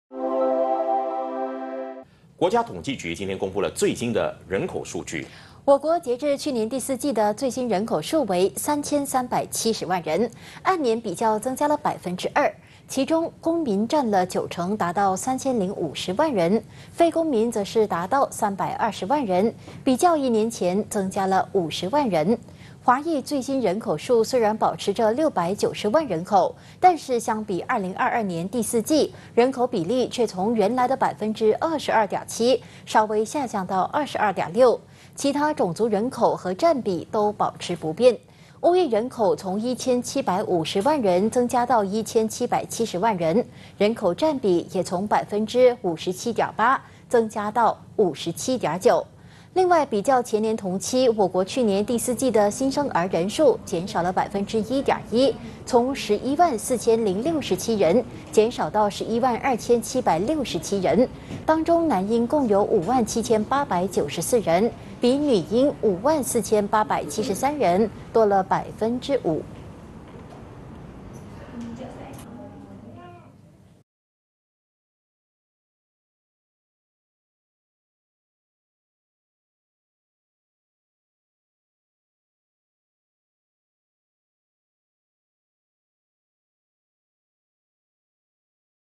最新人口增至3370万 华裔人口占比逐年降【2024.02.14 八度空间华语新闻】
【最新人口增至3370万  华裔人口占比逐年降】
虽然最新华裔人口数仍然保持在690万人，但是人口比例却在下降。

【今日主播】陈嘉荣 & 周秀洋